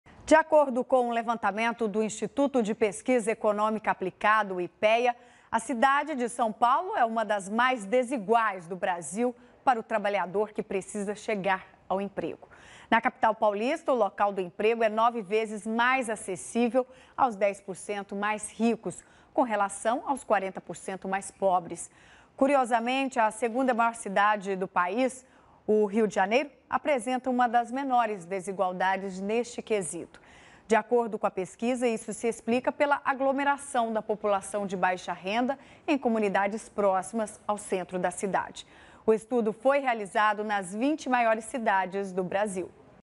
São Paulo é uma das cidades mais desiguais no acesso ao emprego
Do ponto de vista do acesso ao emprego, a cidade de São Paulo é uma das mais desiguais do Brasil. O dado está no diagnóstico da desigualdade de acessibilidade nas 20 maiores cidades brasileiras apresentado nesta quinta-feira (16) pelo Instituto de Pesquisa Econômica Aplicada (Ipea). Isso significa que o número de empregos acessíveis aos 10% mais ricos é nove vezes maior do que o número de empregos acessíveis aos 40% mais pobres.